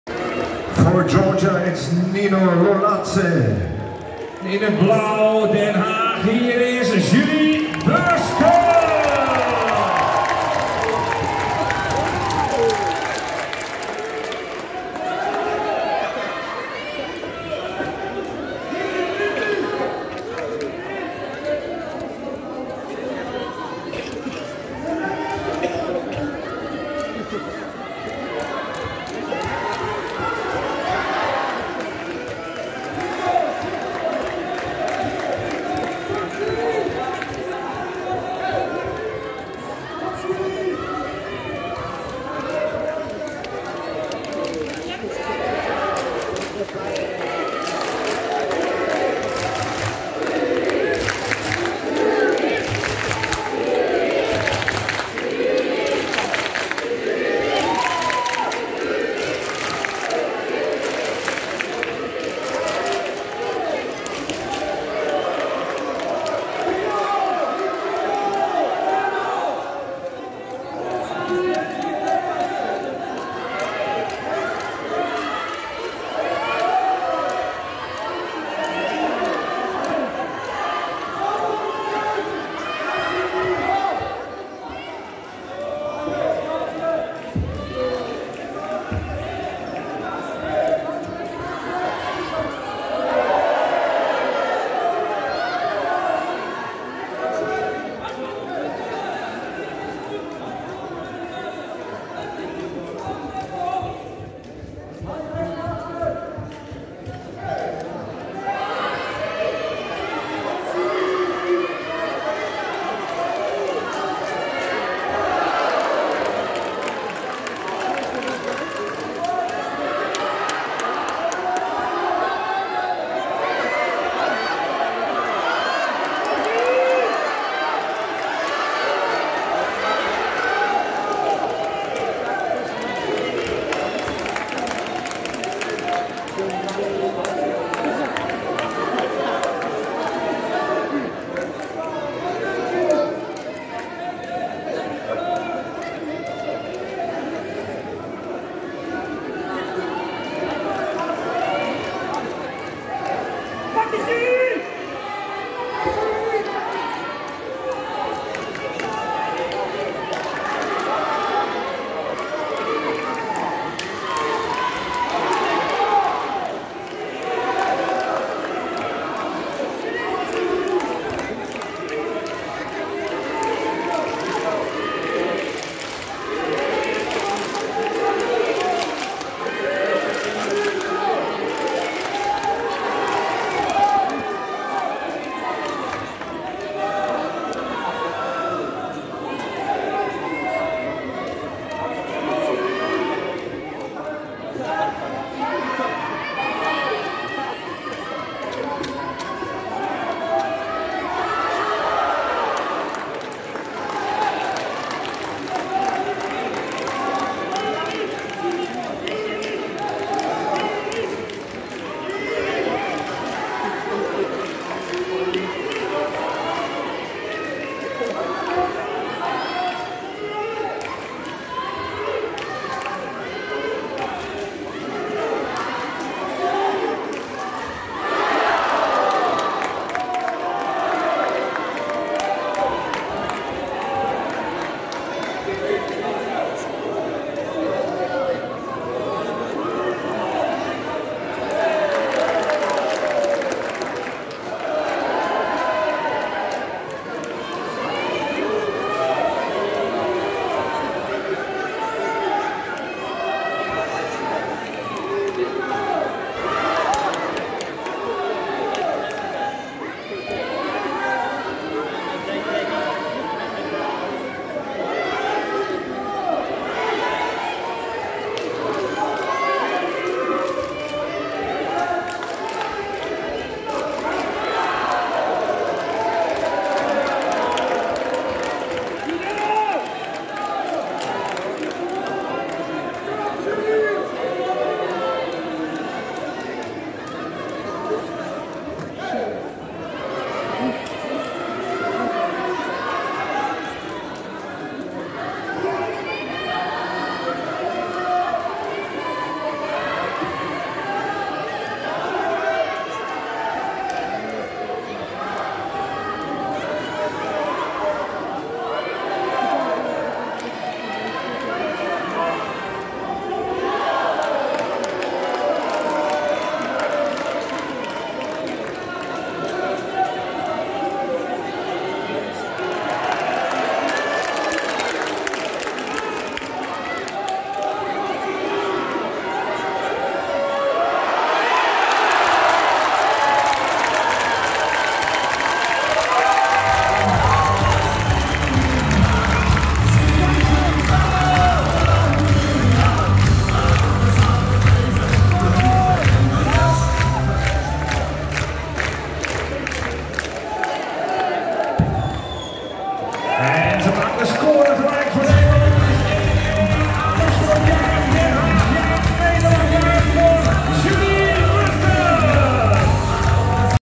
Judo, finals-21 Nederland vs Georgië , Julie Beurskens
Finale teams onder 21 Nederland tegen Georgië . Speler Julie Beurskens
like 👍support 👍comments 👍